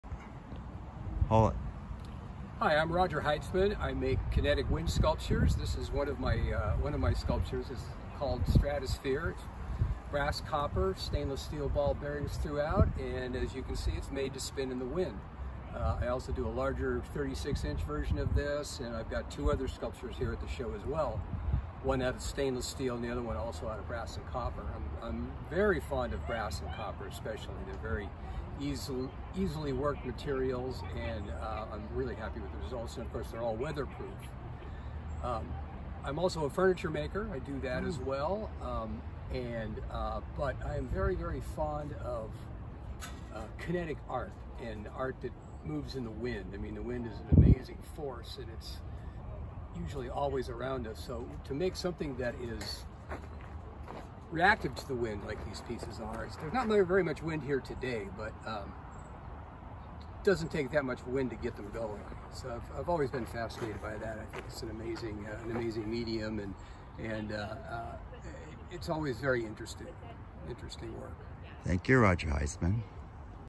Ruth Bancroft Garden Sculpture Show Artist Roger Heitzman talks about his kinetic wind sculptures
Did you know Roger is also a furniture maker? Roger is one of the featured artists in the Ruth Bancroft Garden's 26th Annual Sculpture Show and art sale which takes place at the Garden every summer.  The show marries stunning sculptural art with the natural sculptural beauty of the Garden. This year’s show will feature the works of over 15 renowned Northern California artists who work in a variety of mediums.
Filmed by Milton Schuler